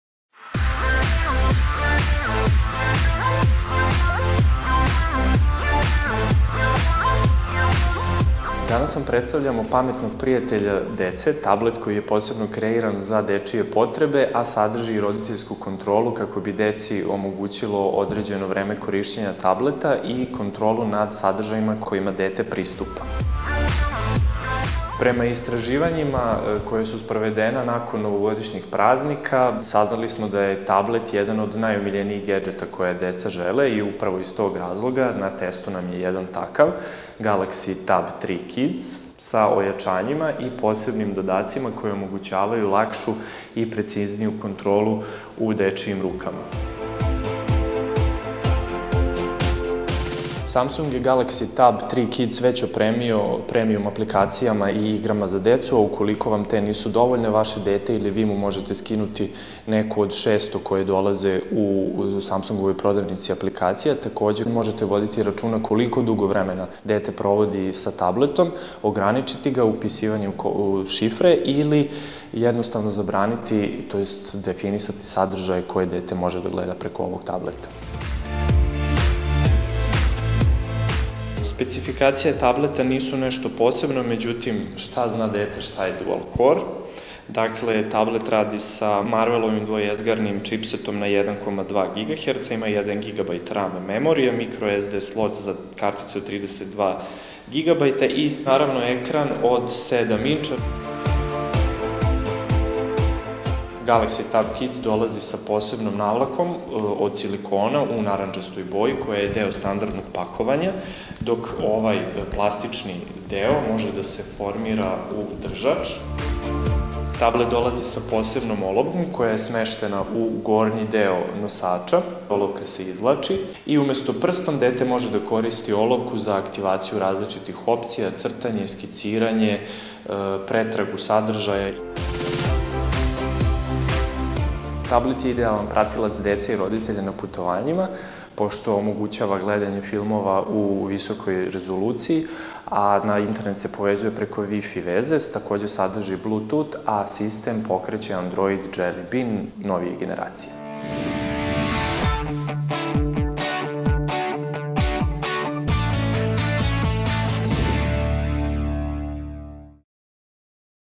Samsung Galaxy Tab 3 Kids - recenzija
Posebno osmišljen tablet za najmlađe nudi im igre i edukativne sadržaje, a roditeljima omogućava bolju kontrolu sadržaja dostupnih detetu - Samsung Galaxy Tab 3 Kids prodaje se u Srbiji po ceni od oko 25.000 dinara.